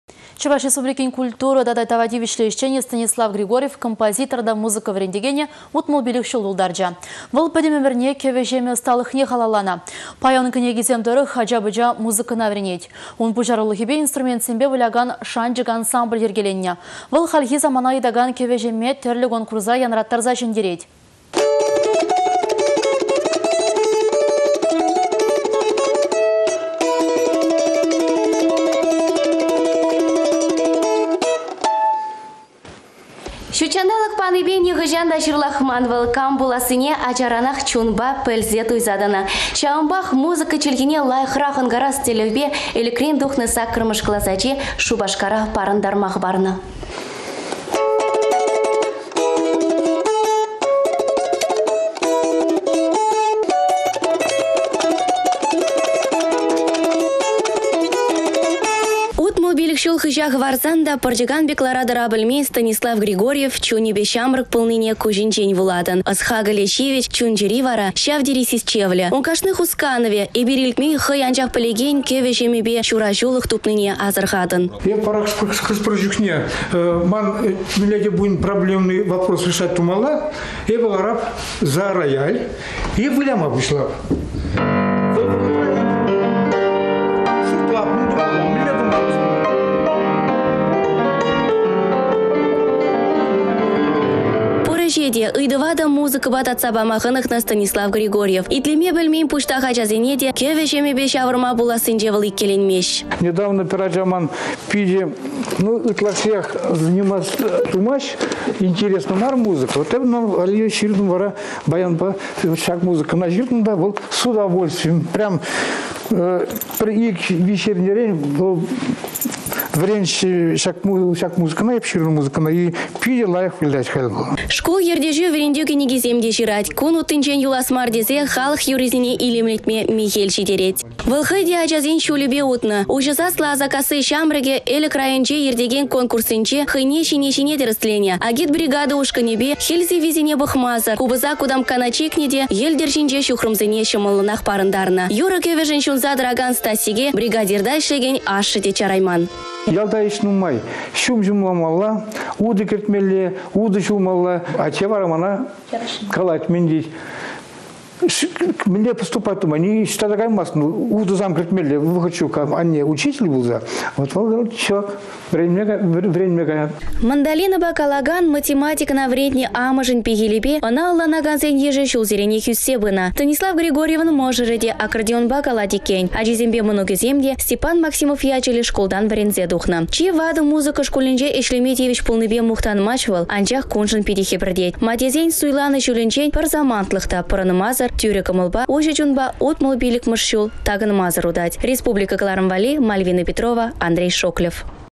Станислав Григорьев -65 çулта
Чăваш Республикин  культурăн тава тивĕçлĕ  ĕçченĕ Станислав Григорьев   композитор тата музыка вĕрентекенĕ 65 çул тултарчĕ.  Вăл пĕтĕм ĕмĕрне кĕвĕ-çемĕ ăсталăхне халалланă. Паян ун кĕнекисем тăрăх ача-пăча музыкăна вĕренет. Ун пуçарулăхĕпе инструментсемпе вылякан «Шанчăк»  ансамбль  йĕркеленнĕ. Вăл хальхи самана ыйтакан кĕвĕ-çемме тĕрлĕ конкурсра янраттарса çĕнтерет.